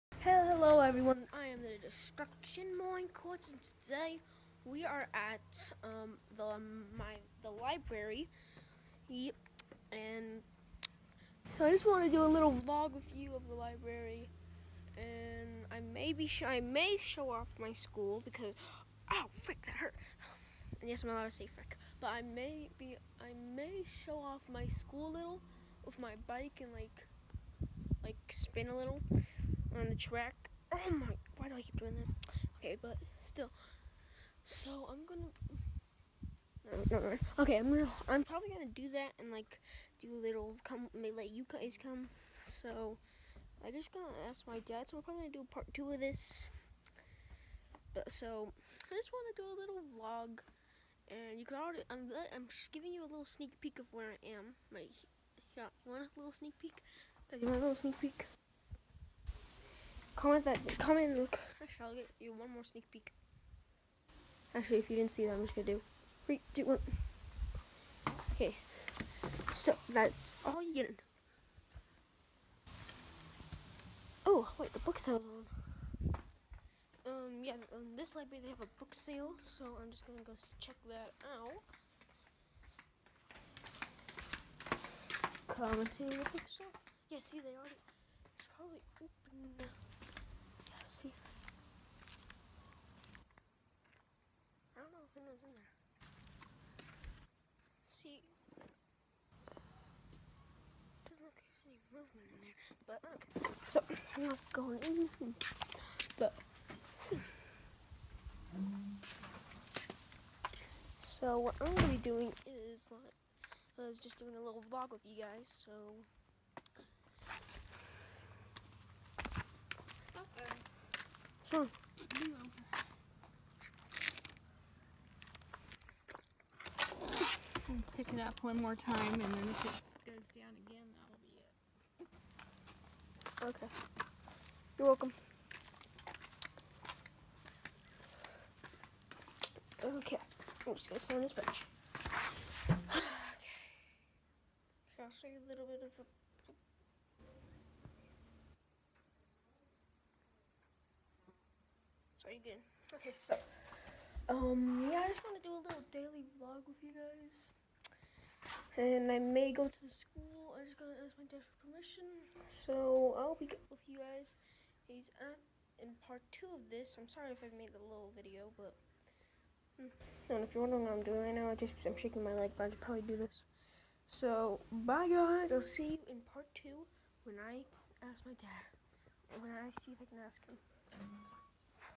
Library 📚 day Part 1
I go to the library to have a little vlog